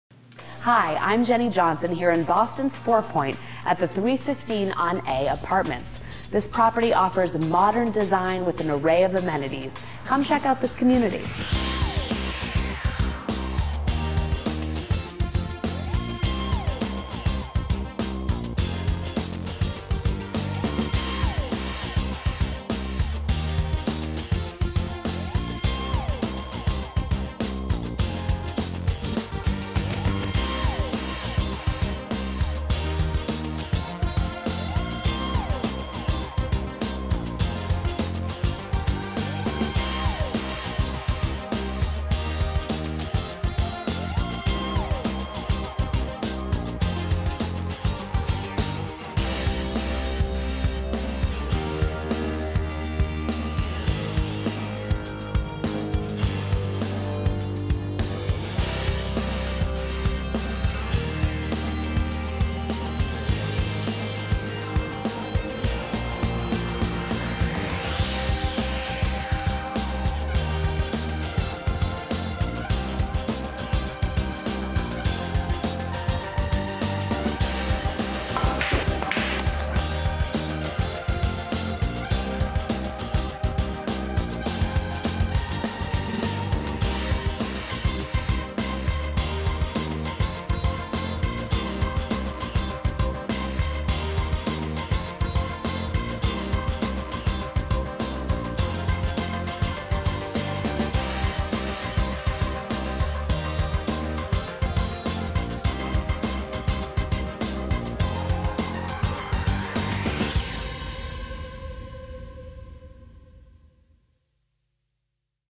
Introducing 315 on A Apartments in Boston
315 on A Apartments was constructed on the site formerly used by the Dwinell-Wright Company, one of the largest and most prominent coffee pioneers throughout the late 1800s. Today, this LEED Certified community is not just environmentally-friendly but also one of the most visually stunning. Stainless steel accents the gorgeous flooring and unique interior layouts. Our rooftop terrace and sky lounge offer clear vantage of the Boston skyline. Residents can entertain in the lounge or game room, work out in the fitness center or explore the neighborhood's collection of shopping, dining and entertainment scenes. Let the breathtaking homes at 315 on A Apartments foster your modern and luxurious lifestyle.